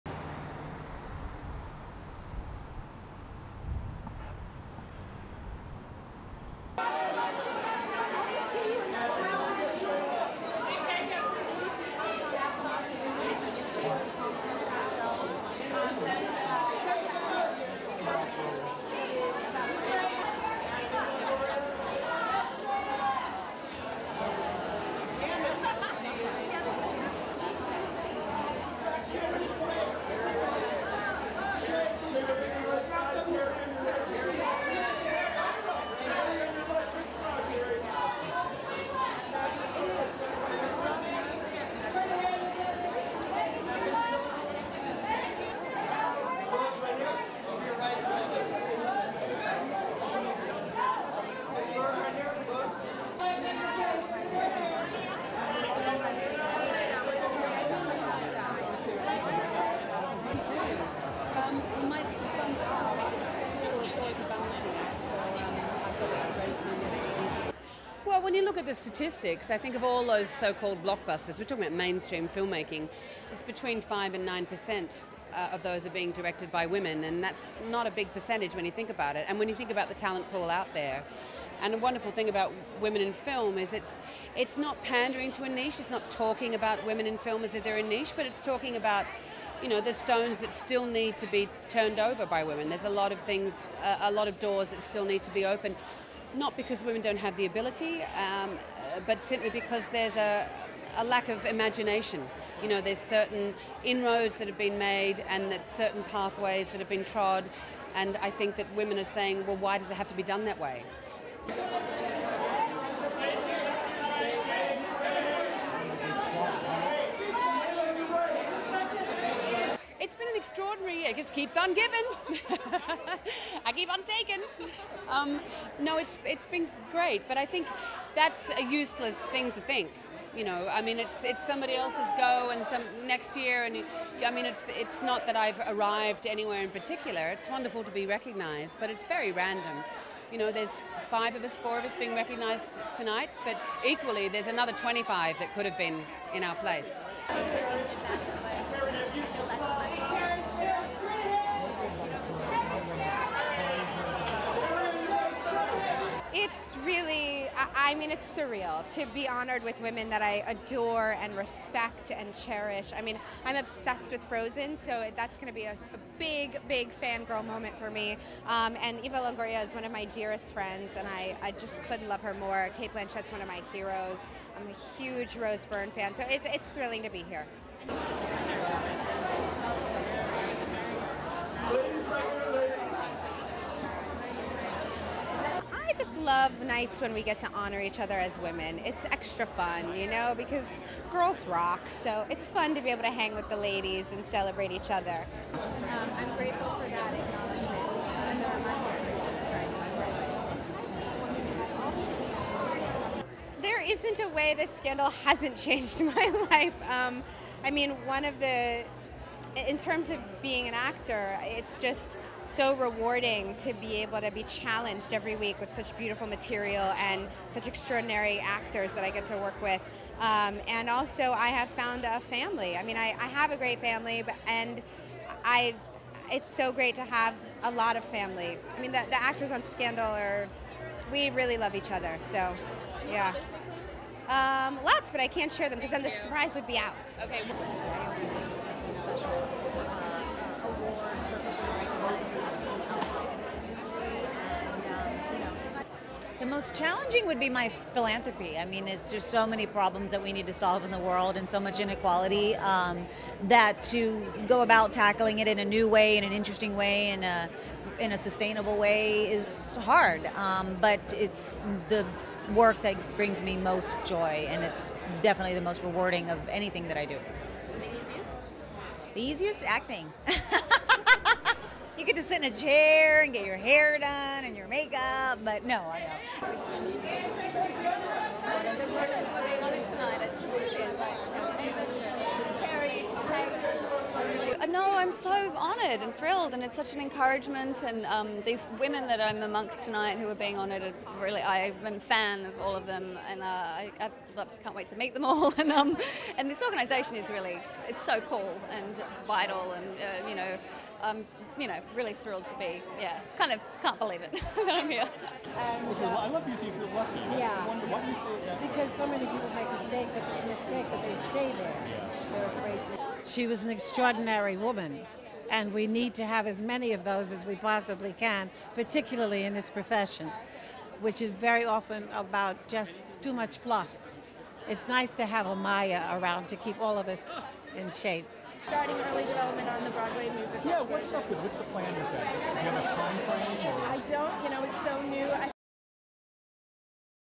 Cate Blanchett, Kerry Washington honored at the Crystal + Lucy Awards
(12 Jun 2014) CATE BLANCHETT ON WOMEN IN SHOWBIZ: 'THERE'S A LOT OF DOORS THAT STILL NEED TO BE OPENED'
Cate Blanchett says there are still strides to be made on the path to gender equality in Hollywood.
"Well when you look at the statistics I think of all those so-called blockbusters, which are mainstream filmmaking, it's between five and nine percent of those are being directed by women and that's not a big percentage when you think about it and when you think about the talent pool out there," she said.
"And the wonderful thing about Women in Film is it's not pandering to a niche. It's not talking about women in film as if they are a niche. It's talking about the stones that still need to be turned over by women. There's a lot of doors that still need to be opened, not because women don't have the ability but simply because there's a lack of imagination you know. There's certain inroads that have been made and certain pathways that have been trod and I think that women are saying 'well why does it have to be done that way?'"
The Academy Award-winning Australian actress was the woman of the hour Wednesday (11 JUNE 2014) at Women in Film's annual Crystal + Lucy Awards. On the red carpet Blanchett, who won the Crystal Award for Excellence in Film, discussed the pressure to follow up her massively successful year.
"It's been an extraordinary year. It keeps on giving. I keep on taking! No, it's been great, but I think that's a useless thing to think you know. I mean it's somebody else's go next year. It's not that I arrived anywhere particular. It's wonderful to be recognized, but it's very random you know. There's five of us, four of us being recognized tonight, but equally there's another 25 that could have been in our place."
New mom Kerry Washington was on hand to accept the 2014 Lucy Award for Excellence in Television.
"I just love nights when we get to honor each other as women. It's extra fun you know because girls rock. So it's fun to be able to hang with the ladies and celebrate each other," she said.
Washington discussed finding a family on the set of her hit ABC series "Scandal."
"There isn't a way that 'Scandal' hasn't changed my life. I mean one of, in terms of being an actor, it's just so rewarding to be able to be challenged every week with such beautiful material and the extraordinary actors that I get to work with. And also I have found a family. I have a great family and it's so great to have a lot of family. The actors of 'Scandal' are, we really love each other."
Washington and retired NFL player Nnamdi Asomugha (NAHM'-dee AH'-suhm-wah) welcomed daughter, Isabelle Amarachi Asomugha, on April 21.
But the famously private starlet kept mum about her father's day plans.
"Lots! But I can't share them because then the surprise would be out," she said.
Actress and producer Eva Longoria was honored for her charity work.
The "Desperate Housewives" star said her humanitarian efforts are the most challenging and most rewarding part of her life.
"The most challenging would be my philanthropy. I mean there's just so many problems that we need to solve in the world and so much inequality that to go about tackling it in a new way, and interesting way, in a sustainable way, is hard. But it's the work that brings me the most joy and it's definitely the most rewarding of anything that I do," she said before heading inside to accept the Norma Zarky Humanitarian Award.
"The easiest?" she continued. "Acting! You get to sit in the chair here and get your hair done and your make up."
Rose Byrne received The Women In Film Max Mara "Face of the Future" award.